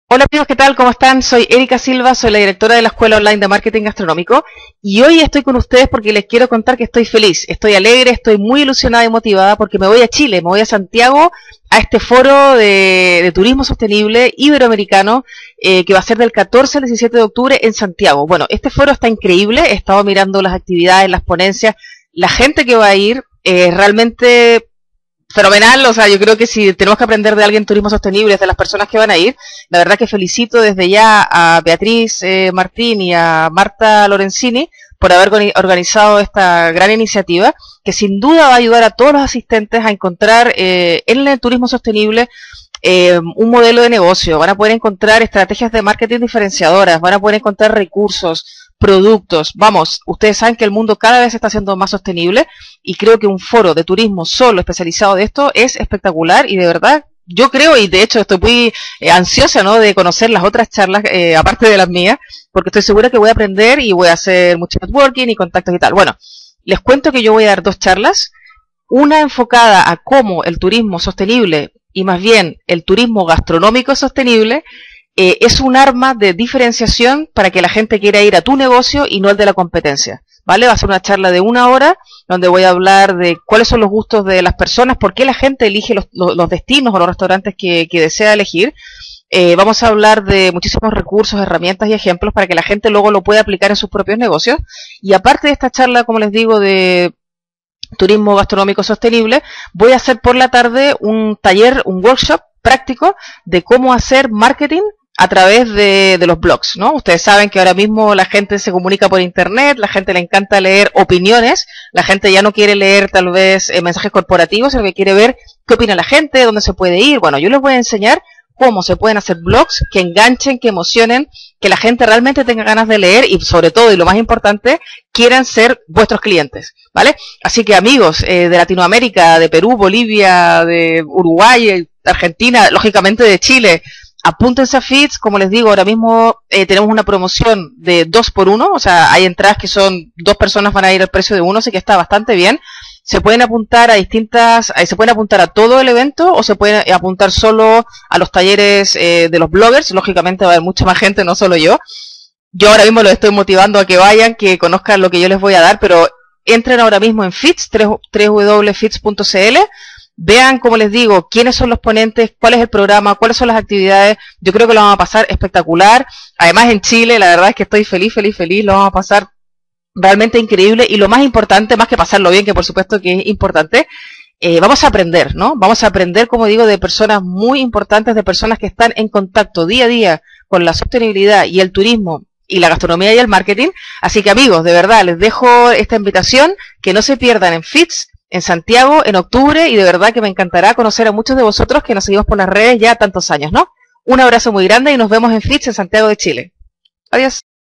Charlas Internacionales de Marketing Gastronómico
Cada año viajo a Sudamérica a dar charlas de marketing gastronómico. Esta fue en Chile.

¿Quieres que participe de ponente en alguno de tus eventos? Mira cómo te puedo ayudar aquí

Como directora de la Escuela de Hostelería Online de Marketing Gastronómico, es un orgullo estar como ponente en este Foro Iberoamericano de Turismo Sostenible en Chile.